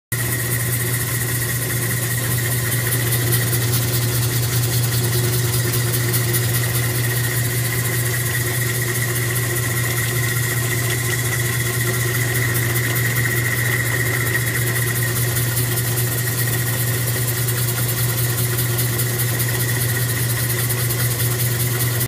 12" Lortone Rock Saw at Work
Cutting Oolite Jasper Collected in Aug. 2015 by my friend Joel & myself.